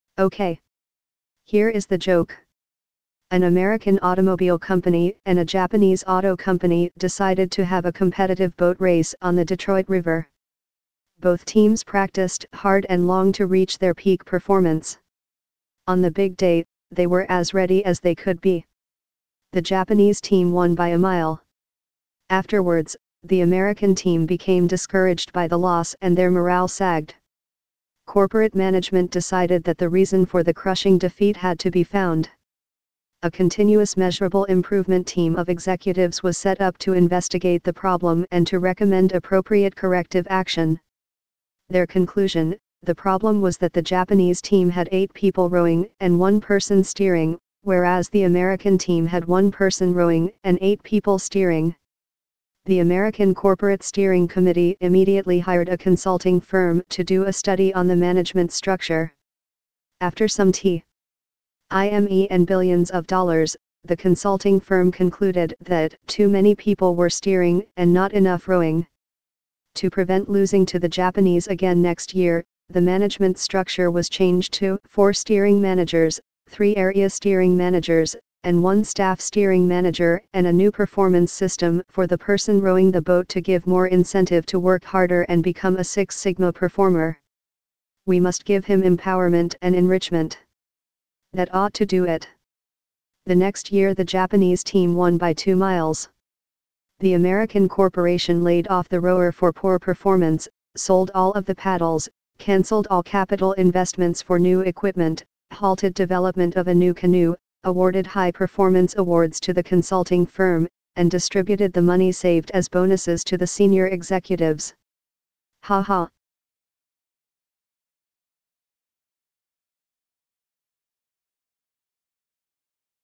Ok, here is the joke. An American automobile company and a Jap...
An American automobile company and a Japanese auto company decided to have a competitive boat race on the Detroit River. Both teams practiced hard and long to reach their peak performance. On the big day, they were as ready as they could be.

The Japanese team won by a mile.

Afterwards, the American team became discouraged by the loss and their morale sagged. Corporate management decided that the reason for the crushing defeat had to be found. A Continuous Measurable Improvement Team of "Executives" was set up to investigate the problem and to recommend appropriate corrective action.

Their conclusion: The problem was that the Japanese team had 8 people rowing and 1 person steering, whereas the American team had 1 person rowing and 8 people steering. The American Corporate Steering Committee immediately hired a consulting firm to do a study on the management structure.

After some t
 ime and billions of dollars, the consulting firm concluded that "too many people were steering and not enough rowing." To prevent losing to the Japanese again next year, the management structure was changed to "4 Steering Managers, 3 Area Steering Managers, and 1 Staff Steering Manager" and a new performance system for the person rowing the boat to give more incentive to work harder and become a six sigma performer. "We must give him empowerment and enrichment." That ought to do it.

The next year the Japanese team won by two miles.

The American Corporation laid off the rower for poor performance, sold all of the paddles, cancelled all capital investments for new equipment, halted development of a new canoe, awarded high performance awards to the consulting firm, and distributed the money saved as bonuses to the senior executives.